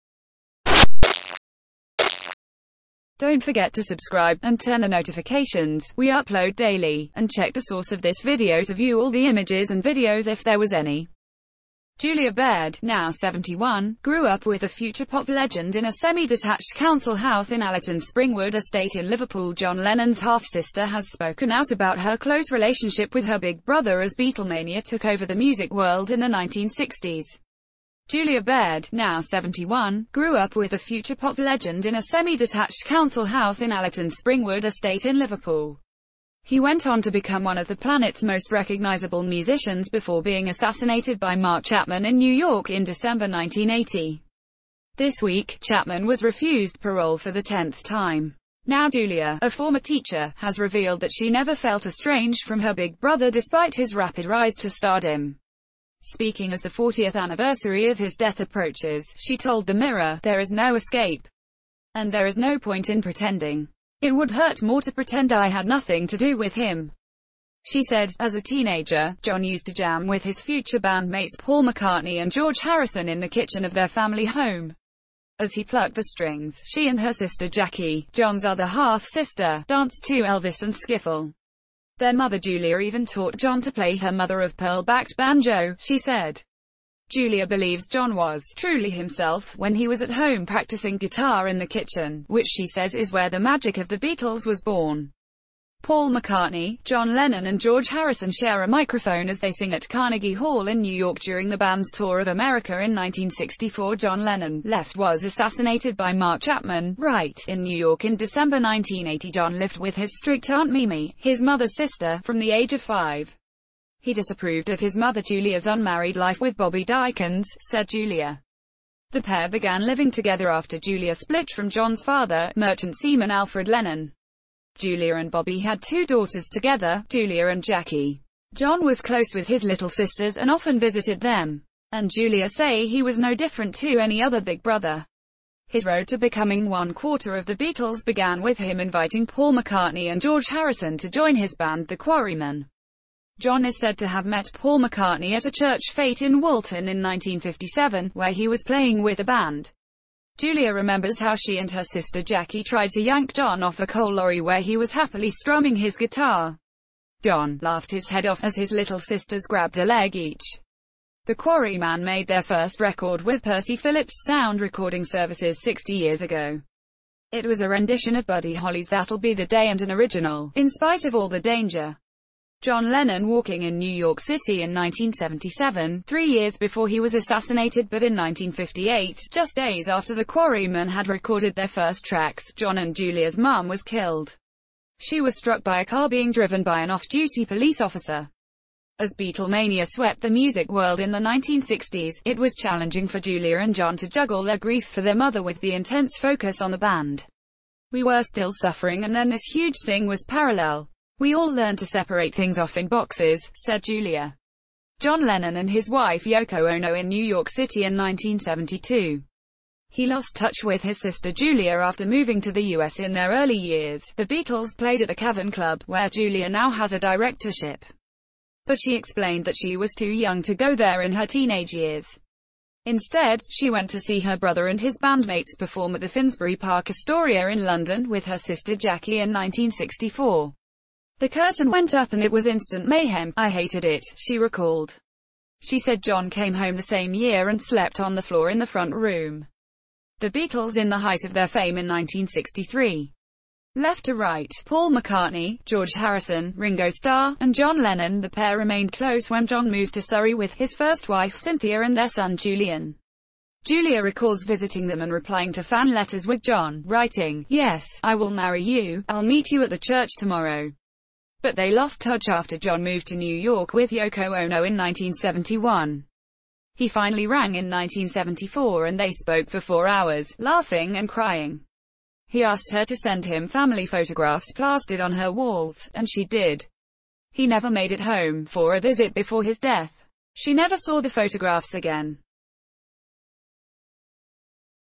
BREAKING! John Lennon’s half–sister reveals how he used to jam in the kitchen of their family home
Source DONT' FORGET TO SUBSCIBE TO OUR CHANNEL Julia Baird, now 71, grew up with the future pop legend in a semi-detached council house in Allerton's Springwood estate in Liverpool

John Lennon's half-sister has spoken out about her close relationship with her big brother as Beatlemania took over the music world in the 1960s.

Julia Baird, now 71, grew up with the future pop legend in a semi-detached council house in Allerton's Springwood esta ...